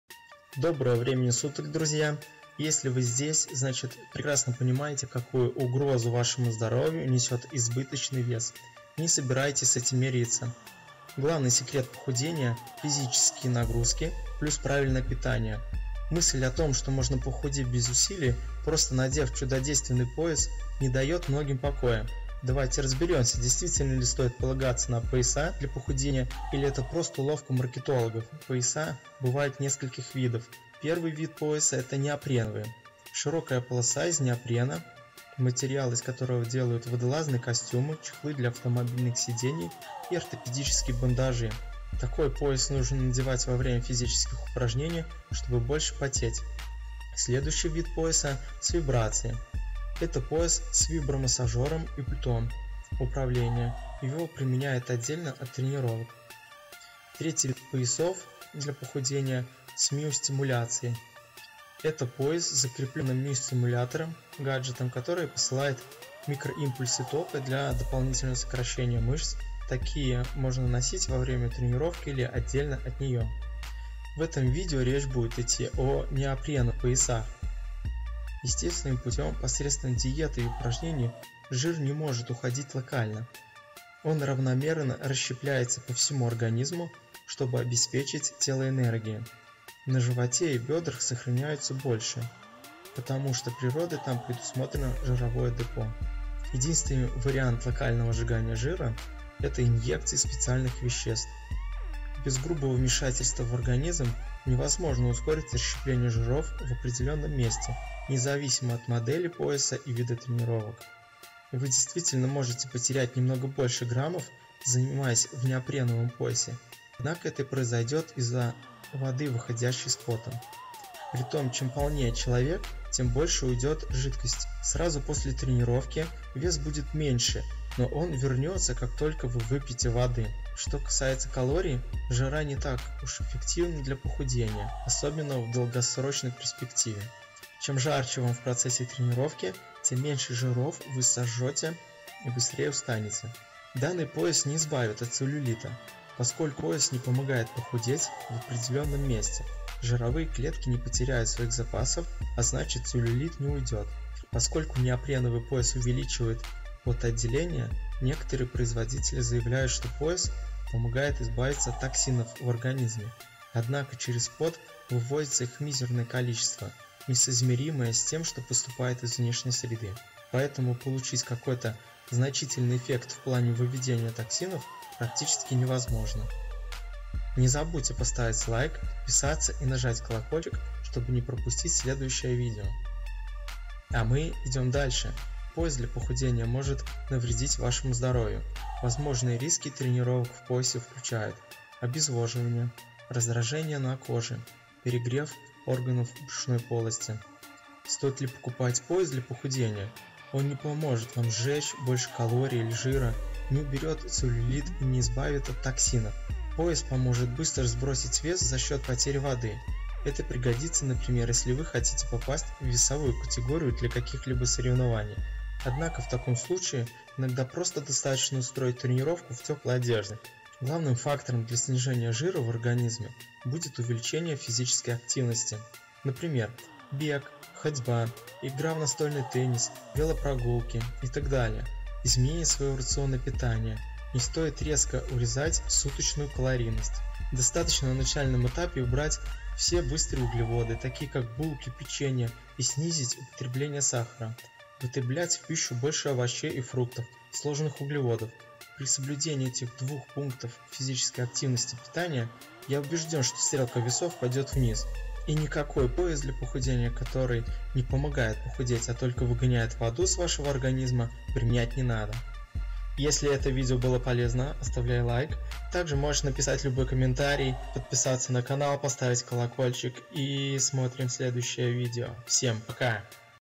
Пояс для похудения живота. Пояс сауна для похудения. Калистеника. Спорт. Фитнес.
В этом выпуске речь пойдет о поясах для похудения, их разновидностях, и возможностях различных марок поясов. Пояса бывают нескольких видов, неопреновые, с вибрацией и с миостимуляцией. У каждого вида пояса своя функция. Может ли помочь сжечь жир, пояс для похудения живота. Досмотрев это видео до конца, узнаешь возможно ли вообще похудеть с помощью таких поясов.

Неопреновый пояс (бандаж) — специальный пояс из неопреновой ткани, который применяется во время занятий спортом для ускорения процесса похудения в области живота.
Пояс с вибрацией. Это пояс с вибромассажёром и пультом управления. Его применяют отдельно от тренировок.
Пояс с миостимуляцией (EMS). Это пояс с закреплённым миостимулятором — гаджетом, который посылает микроимпульсы тока для дополнительного сокращения мышц. Такие можно носить во время тренировки или отдельно от неё.

Ускоряет ли пояс для похудения расщепление жира на талии
Естественным путём — посредством диеты и упражнений — жир не может уходить локально. Он равномерно расщепляется по всему организму, чтобы обеспечить тело энергией. На животе и бёдрах сохраняется дольше, потому что природой там предусмотрены жировые депо.

Единственный вариант локального сжигания жира — это инъекцииLocal fat treatments: classification proposal специальных веществ, например метилксантинов, аналогов тиреоидных гормонов или селективных альфа-2 адреноблокаторов. Эти препараты запускают каскад реакций для расщепления жиров вне зависимости от того, что происходит в организме в целом. Также применяется локальное разрушение жировых клеток и липосакция — удаление жира.

Без грубого вмешательства в организм невозможно ускорить расщепление жиров в определённом месте — независимо от модели пояса и вида тренировок.

Помогает ли пояс для похудения сбросить вес в целом
Вы действительно можете потерять немного больше граммов, занимаясь в неопреновом поясе. Однако это произойдёт из-за водыDoes Sweating Help You Burn More Calories?, выходящей с потом. Притом чем полнее человек, тем больше уйдёт жидкости от перегрева организма.

Сразу после тренировки вес будет меньше, но он вернётся, как только вы попьёте.

Что касается калорий, жара не так уж эффективна для похудения, особенно в долгосрочной перспективе. Например, в бикрам-йоге обычные асаны выполняют в нагретом до 40 °С помещении. Исследование показалоBikram Yoga Training and Physical Fitness in Healthy Young Adults, что 90 минут занятий сжигает только 330 ккал у женщин и 460 — у мужчин. Столько же можно потерять, прогуливаясь полтора часа при обычной погоде.

Другие учёные выяснили, что восемь недель йога-сессий по три раза в неделю не помогаютThe Effects of Bikram Yoga on Health: Critical Review and Clinical Trial Recommendations сбросить жир — разве что пожилым людям с ожирением.

Чем жарче вам в процессе тренировки, тем меньшеAerobic fitness determines whole-body fat oxidation rate during exercise in the heat жиров вы сожжёте и быстрееEffects of ambient temperature on the capacity to perform prolonged cycle exercise in man устанете.

При этом пояс с миостимуляцией может повысить трату калорий. ИсследованиеEffect of Neuromuscular Electrical Muscle Stimulation on Energy Expenditure in Healthy Adults подтвердило, что EMS увеличила затраты энергии на 3–11 ккал в час, однако электроды располагались не только на животе, но и на бёдрах. Но это слишком маленький результат.

Вибрация также иногда повышаетMetabolic effect of bodyweight whole-body vibration in a 20-min exercise session: A crossover study using verified vibration stimulus расход калорий — в среднем на 20%, но для этого нужно тренироваться на вибрационной платформе. Сколько дополнительных калорий сожжёт пояс, в котором вы смотрите телевизор, предсказать сложно.

Укрепляет ли пояс для похудения мышцы
Обычный неопреновый пояс — нет. Занимаетесь вы в нём или без, нагрузка будет одинаковой.